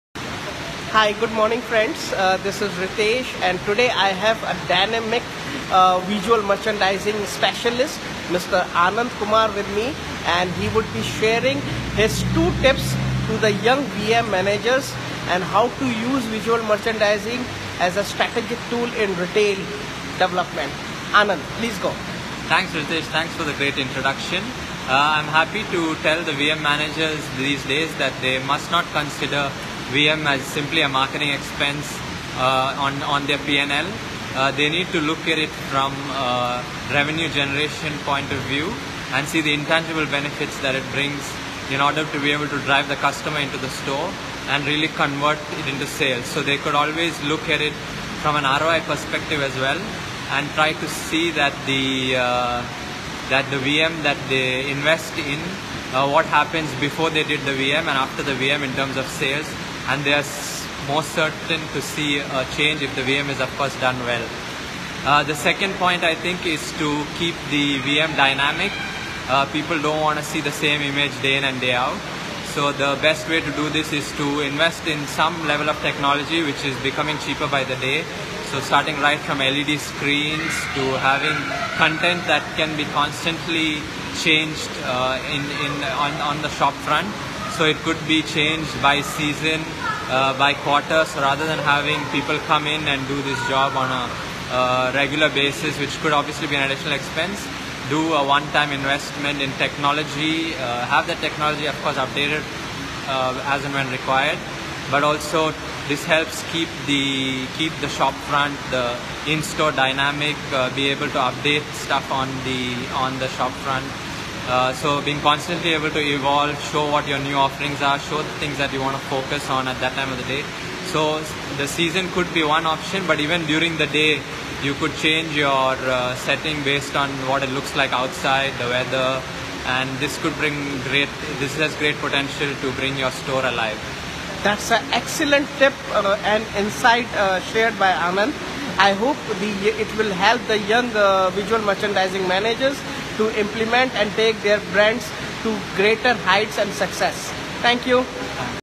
Visual merchandising: A silent salesman in retail
Using Visual merchandising as a strategic tool for brand building.